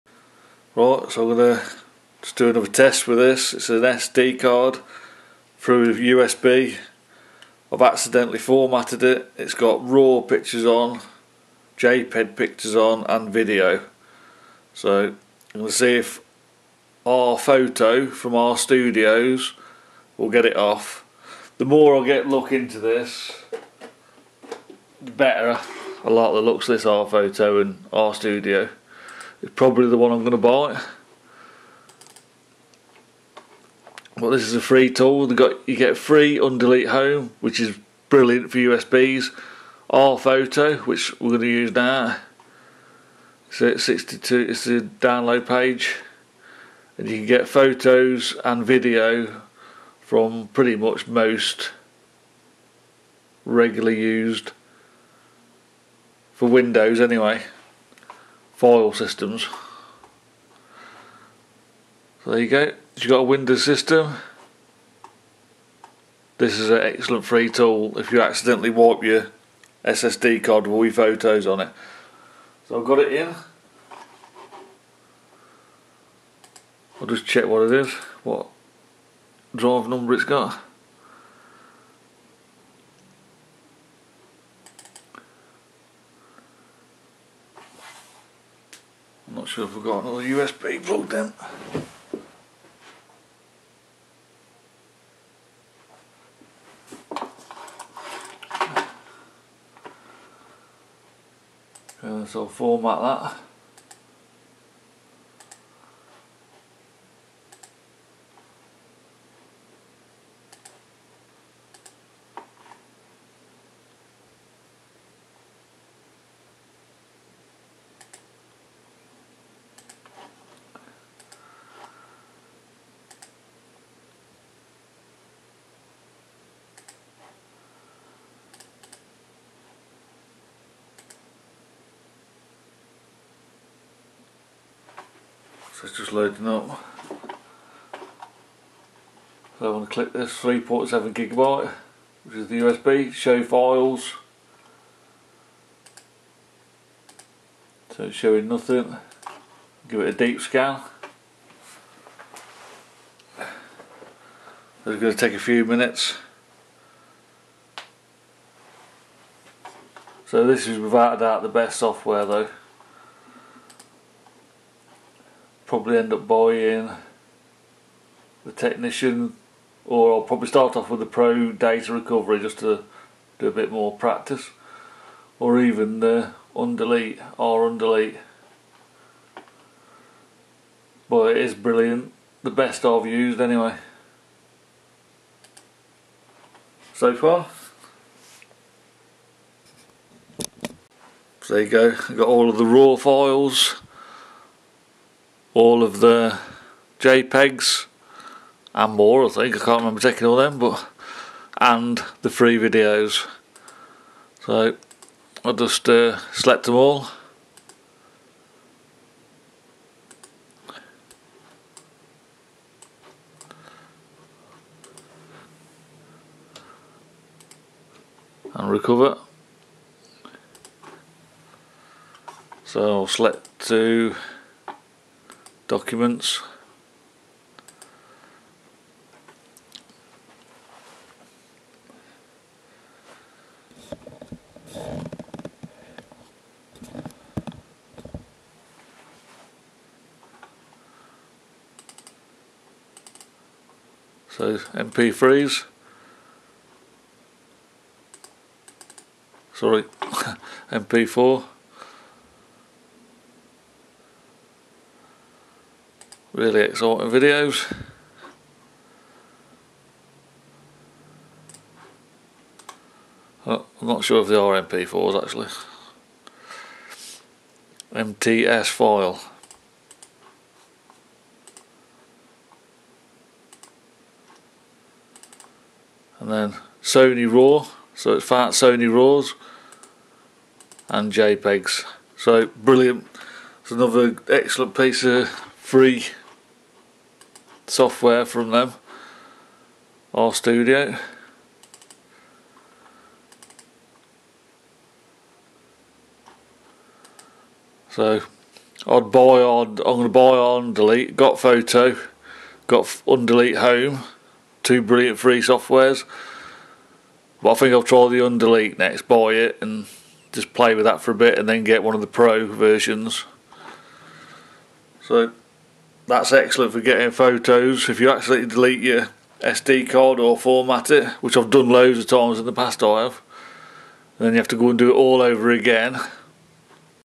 SD Card Photo/Video Recovery: R-Photo JPEG RAW MP4 Tutorial
Lost all your precious photos and videos after a camera or phone SD card failure? This is the definitive tutorial on how to use R-Photo (the free tool for media recovery) and R-Studio (the advanced professional solution) to get them back!

We'll walk you through the most effective method: performing a Deep Scan and using Raw File Recovery (file signatures) to find your lost JPEG, RAW (e.g., CR2, NEF), and MP4 video files, even if the SD card was formatted or corrupted.

🚨 Pro Tip: Always create a Disk Image of your SD card first using R-Studio to prevent further data loss!